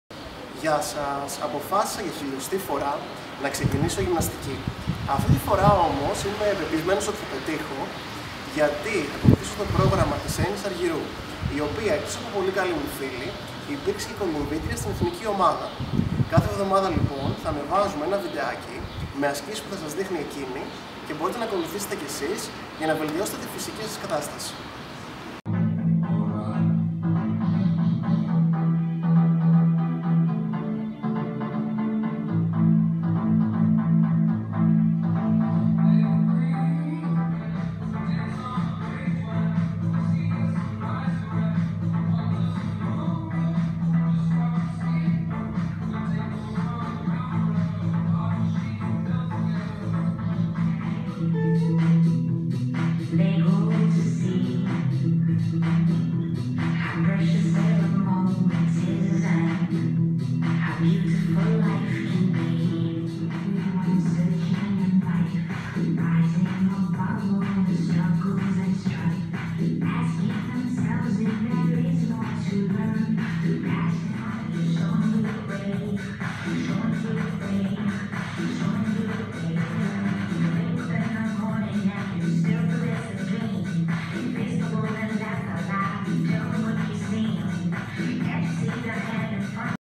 Mister Posh: Training with Eni Argyrou
Introducing our new lifestyle section at poshfashionnews.com with fitness tips and videos featuring Eni Argyrou, a close friend of mine and swimmer at the national greek team. Enjoy the workout!